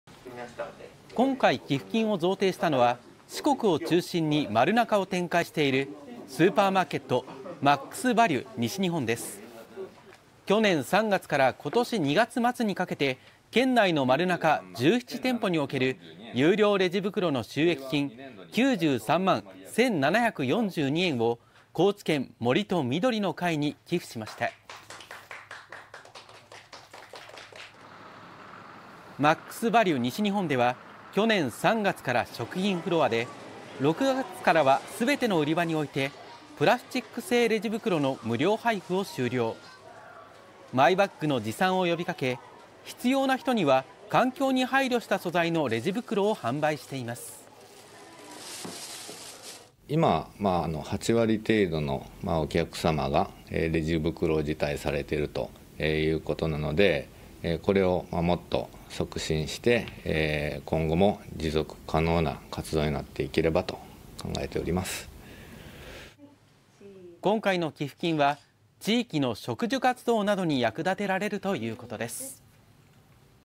森林保全に役立てて 大手スーパーが有料レジ袋の収益金を寄付「持続可能な活動に」【高知】 (21/10/19 19:00)
大手スーパーマーケットが有料レジ袋の収益金を環境保全活動に取り組んでいる団体に寄付しました。

今回、寄付金を贈呈したのは四国を中心に「マルナカ」を展開しているスーパーマーケット「マックスバリュ西日本」です。去年3月から今年2月末にかけて、県内のマルナカ17店舗における有料レジ袋の収益金93万円1742円を『高知県森と緑の会』に寄付しました。

マックスバリュ西日本では去年3月から食品フロアで、6月からは全ての売場においてプラスチック製レジ袋の無料配布を終了。マイバッグの持参を呼びかけ、必要な人には環境に配慮した素材のレジ袋を販売しています。

マックスバリュ西日本　四国営業統括部
浜田正義　高知営業部長
「今、8割程度のお客様がレジ袋を辞退されている。もっと促進して今後も持続可能な活動になっていければ。」

今回の寄付金は地域の植樹活動などに役立てられるということです。